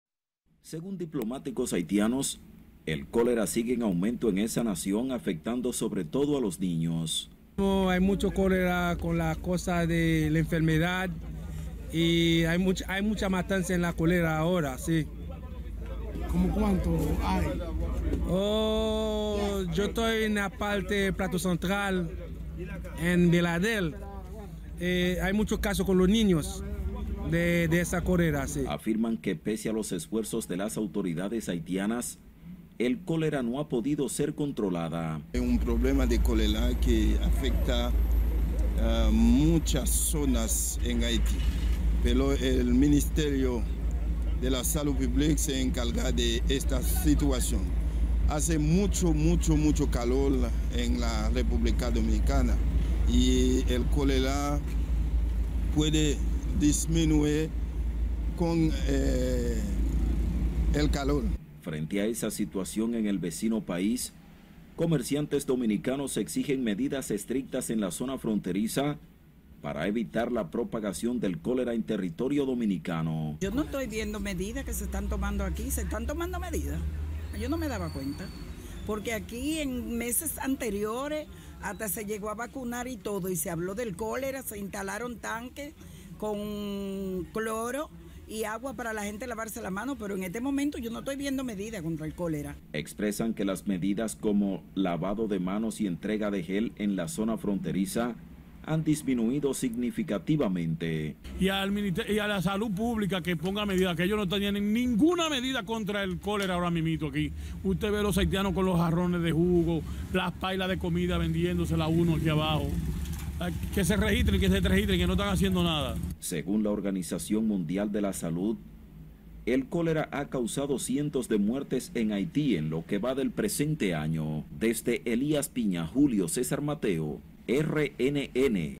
En Elías Piña piden medidas estrictas para evitar propagación del cólera
El aumento de los casos de cólera en Haití mantiene preocupados a funcionarios del área consular de esa nación apostados en Elías Piña, mientras comerciantes en la zona exigen medidas más estrictas que eviten la enfermedad entre a territorio dominicano.